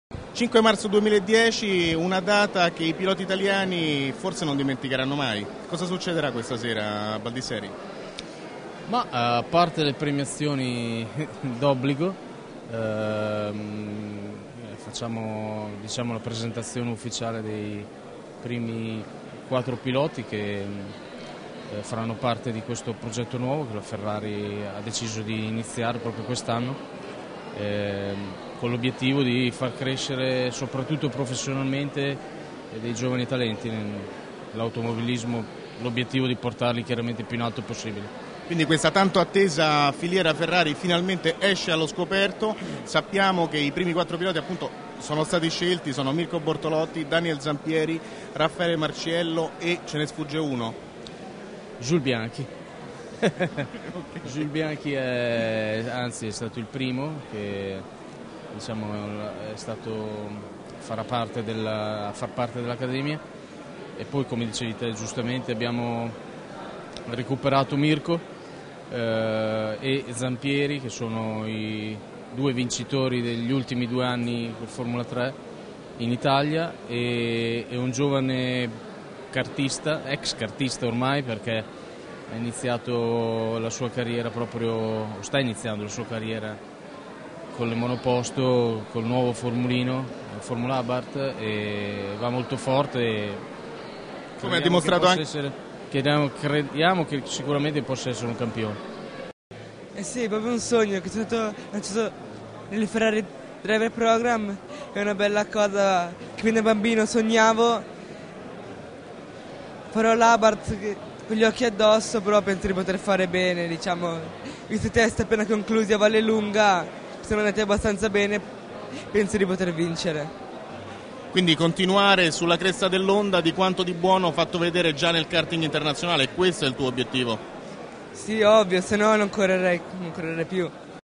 Lello "arruolato" nel Ferrari Driver Academy (FDA)
Intervista a Luca Baldisserri che accoglie Lello nella FDA